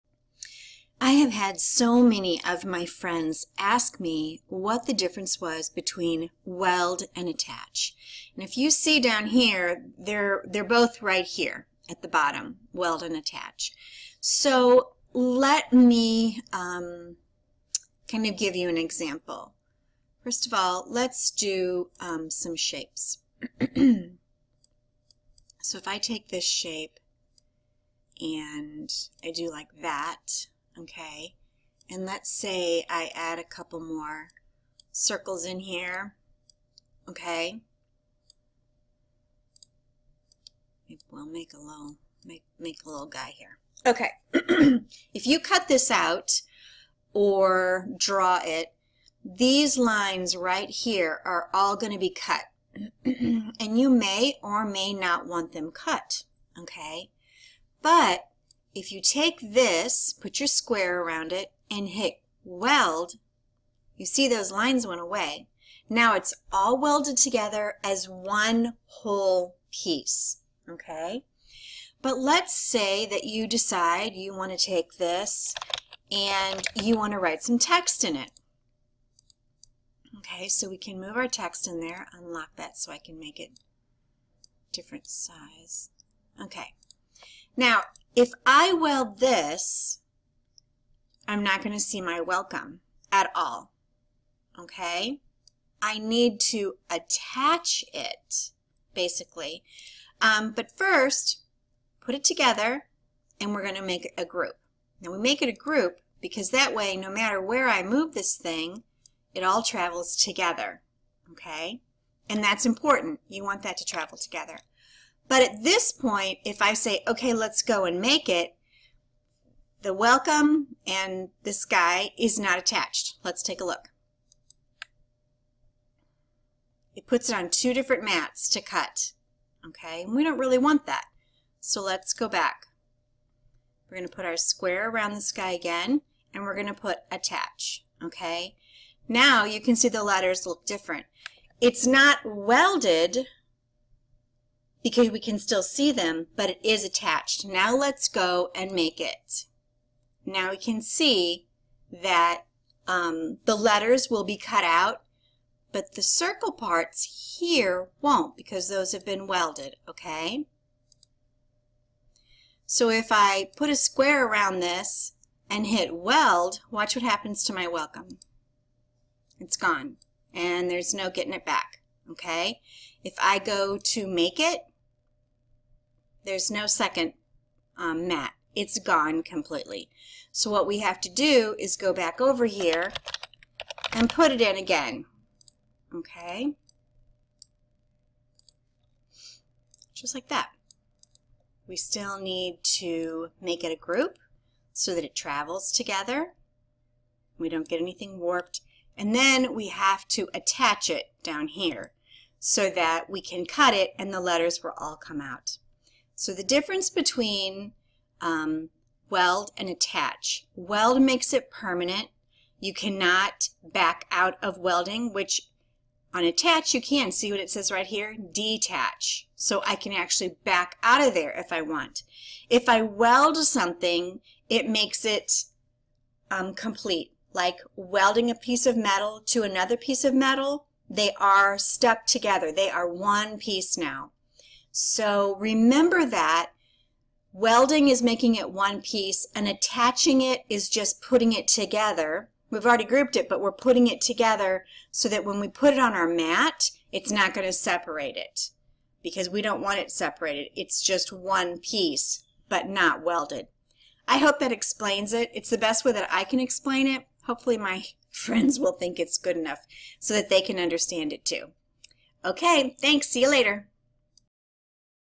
Cricut Weld vs Cricut Attach
What is the difference between weld and attach in Cricut Design Space?

Don't miss our Addicted to Cricut Blog with lots of tutorials and how-tos: